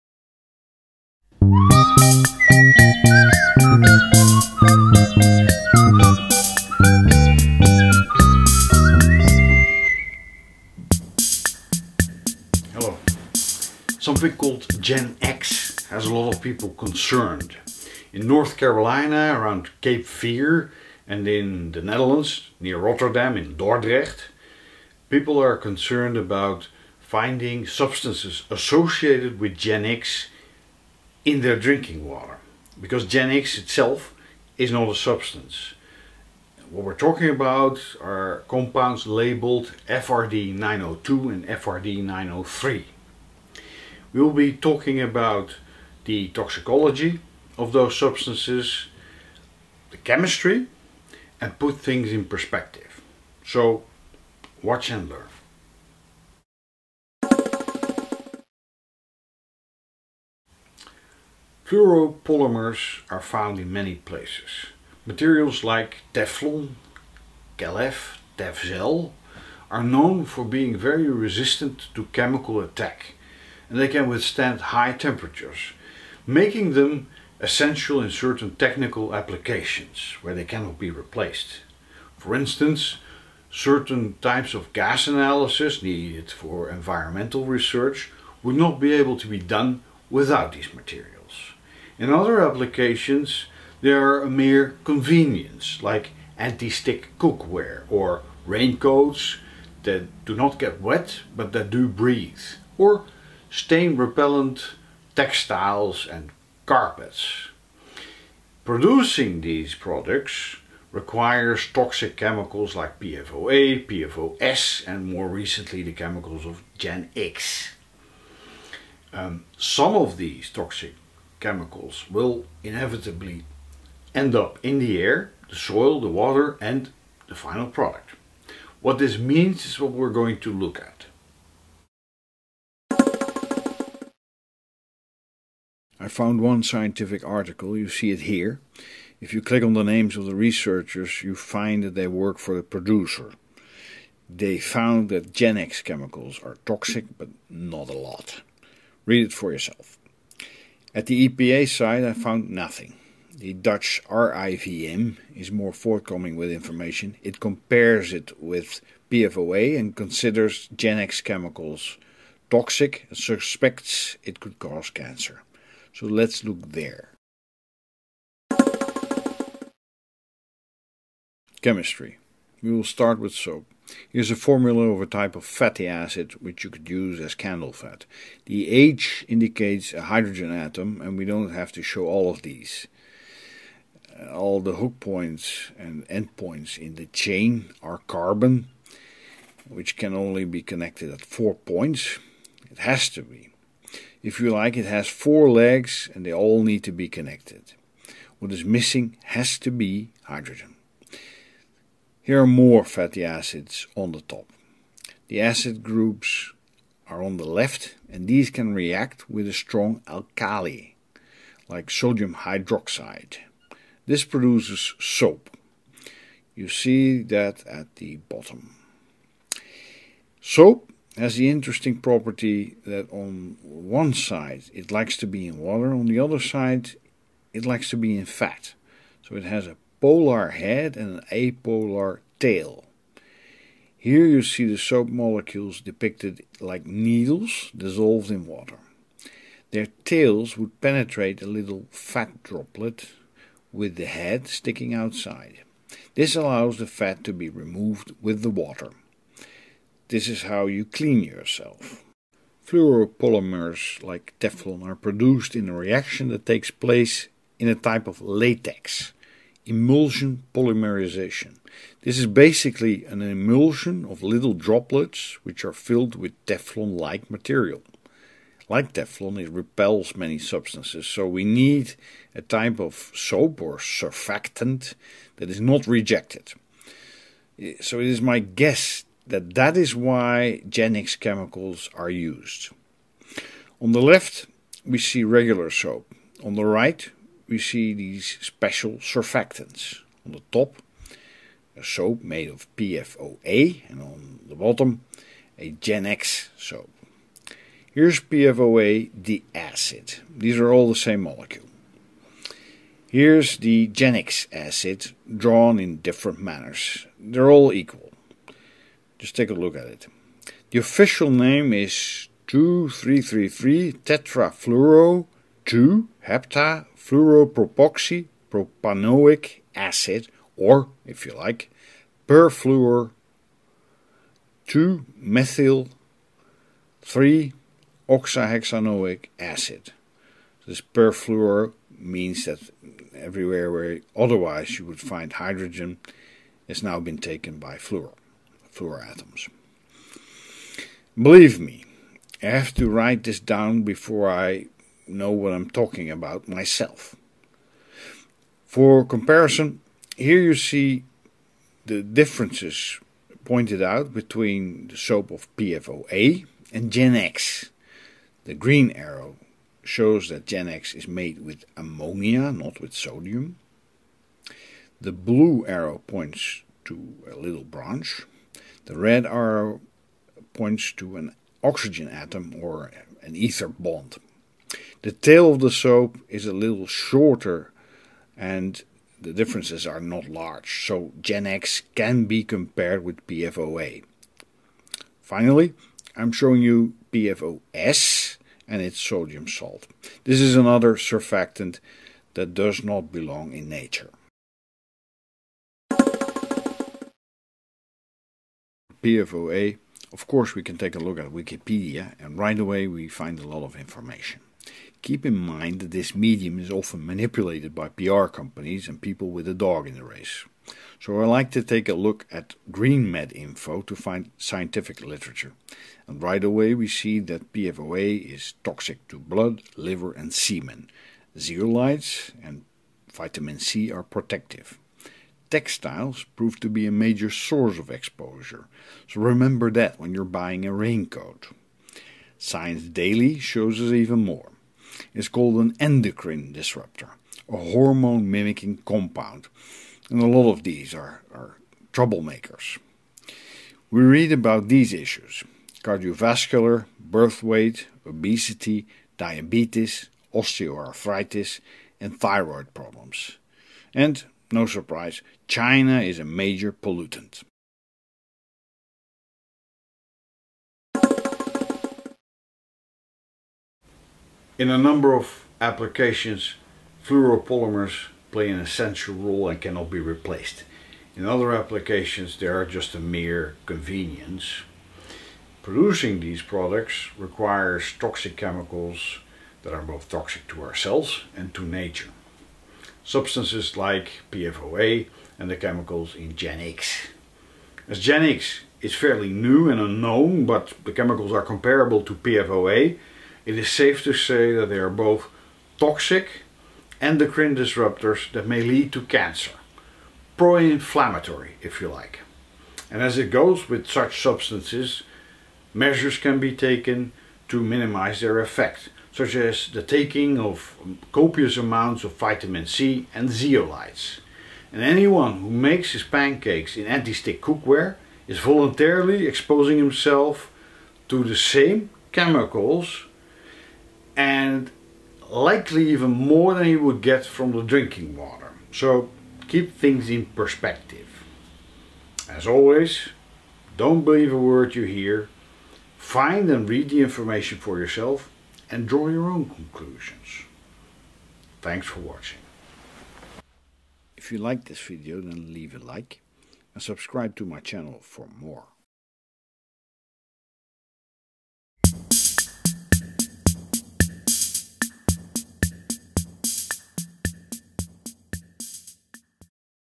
Hendrick solves (58b): GenX & PFOA
GenX technology caused concern with people who find it in their drinking water. The substances involved replaced others, like PFOA, who are considered carcinogens. This episode analyzes what is known about these materials, both their toxicology and chemistry.